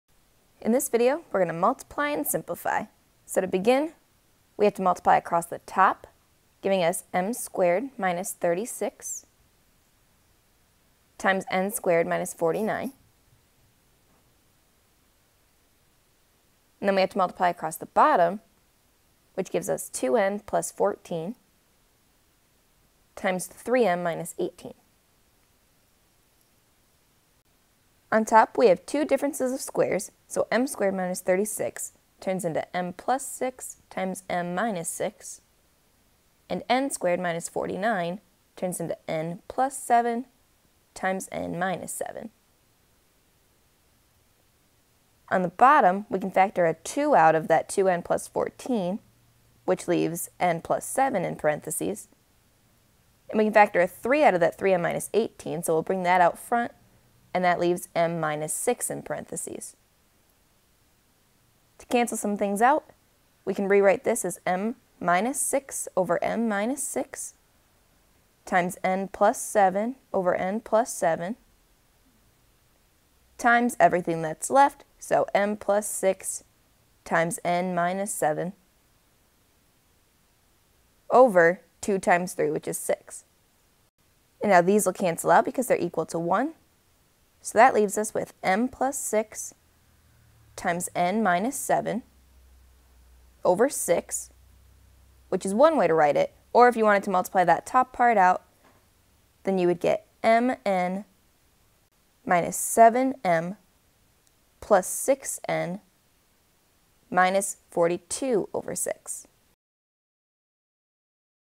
Rational Expressions: Multiply (m^2 - 36)/(2n + 14) * (n^2 - 49)/(3m - 18)
Megan from SVSU Micro Math helps you multiply rational expressions and simplify if possible.  A rational expression is a fraction of polynomials.  The rational expressions in this problem have two variables.

Problem: Multiply (m^2 - 36)/(2n + 14) * (n^2 - 49)/(3m - 18)